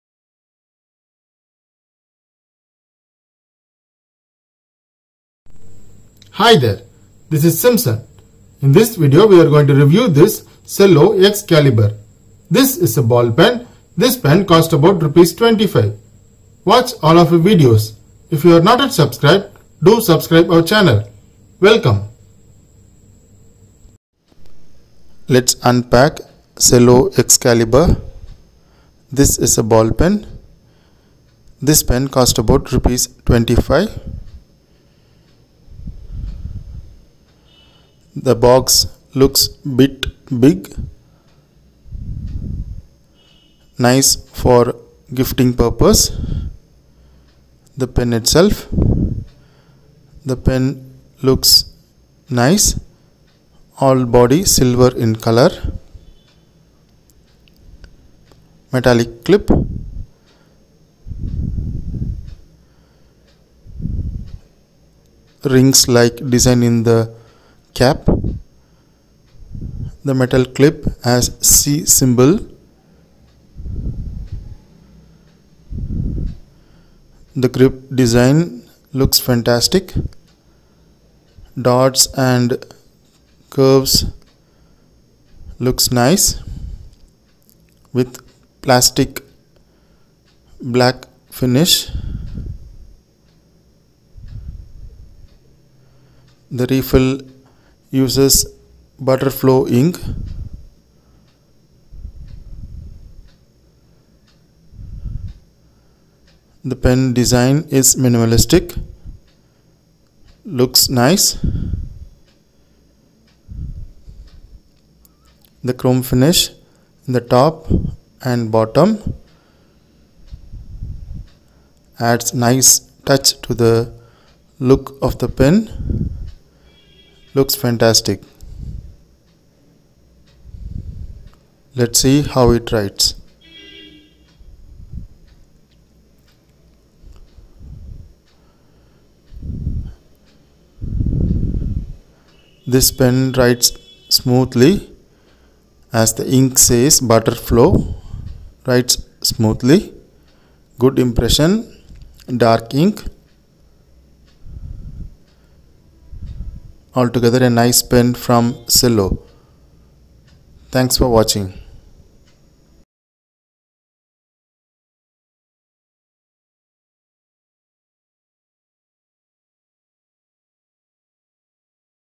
How this pen looks sooo Executive? | cello X CALIBER | Ballpoint Pen
MRP 25 INR | Ballpoint Pen | Medium Tip | Plastic Engrave Grip | Smudge Free | Plastic Body | Metal Clip | Medium Sized Refills | Dark in color